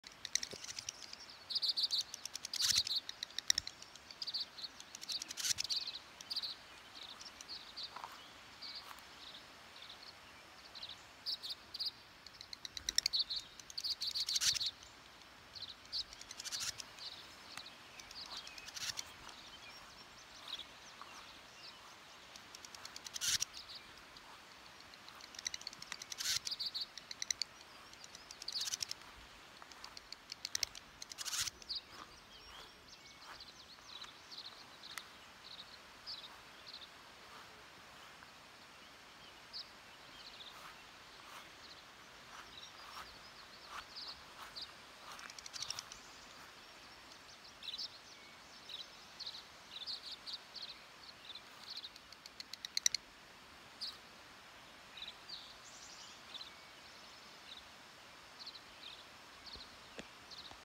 Barn Swallow Attack
Barn swallows dive bombing and swooping over my head.